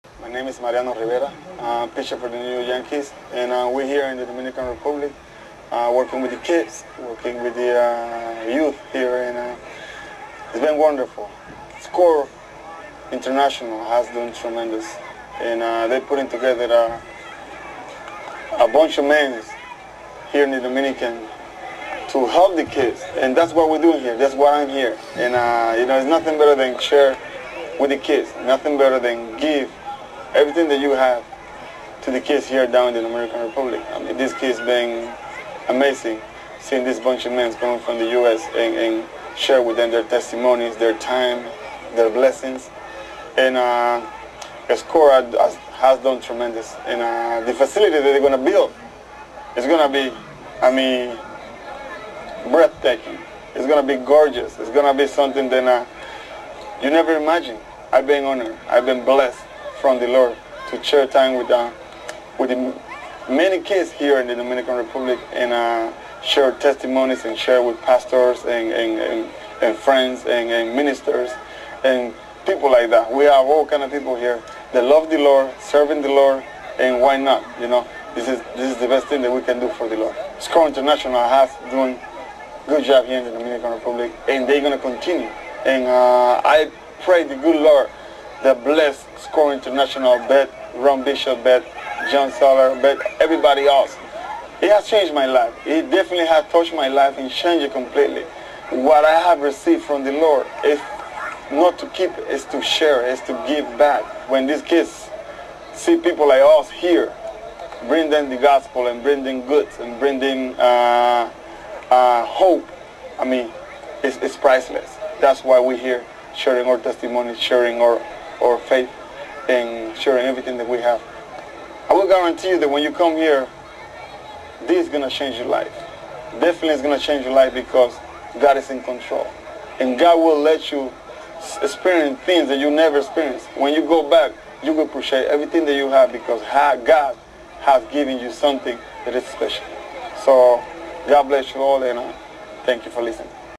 Mariano Rivera
Mariano serves the Lord on a missions trip to the Dominican Republic with SCORE International